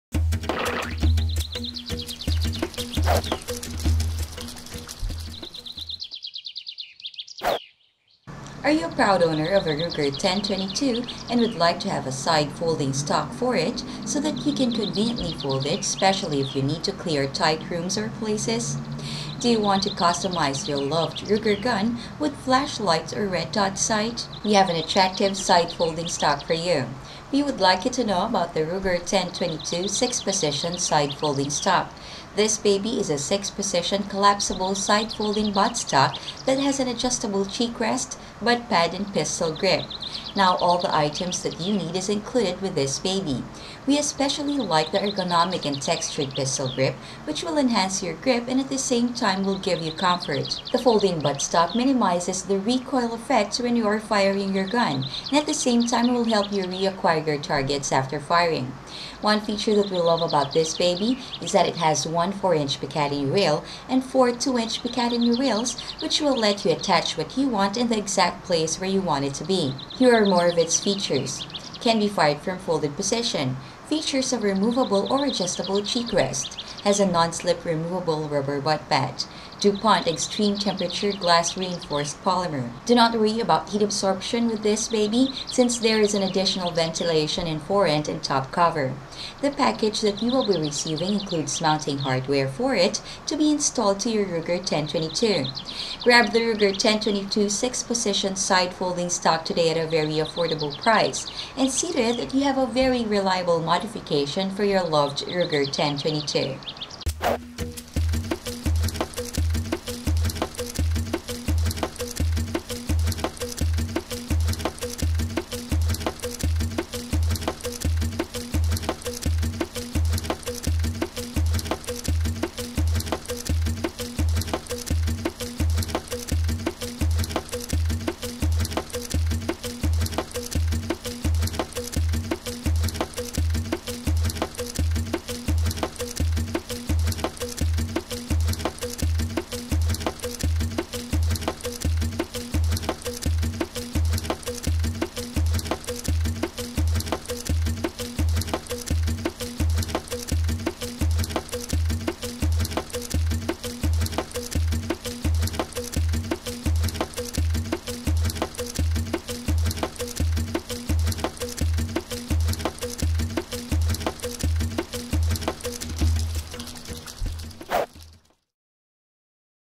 Ruger 10/22 Rifle 6-Position Side - Folding Rifle Stock Review
It is Ruger10/22 6-Position Sd-Fldg Stk; this baby is a 6 Position Collapsible Side Folding Buttstock that has an adjustable cheekrest, buttpad and Pistol Grip.  Now all the items that you need is included with Ruger10/22 6-Position Sd-Fldg Stk; we especially liked the ergonomic and textured pistol grip which will enhance your grip and at the same time will give you comfort.  The folding buttstock minimizes the effect of the recoil effect when you are firing your gun and at the same time will help you reacquire your targets after firing.   One feature that we loved about Ruger10/22 6-Position Sd-Fldg Stk is that it has one 4 inches picatinny rail and four 2 inches picatinny rails which will let you attach what you want in the exact place where you want it to be.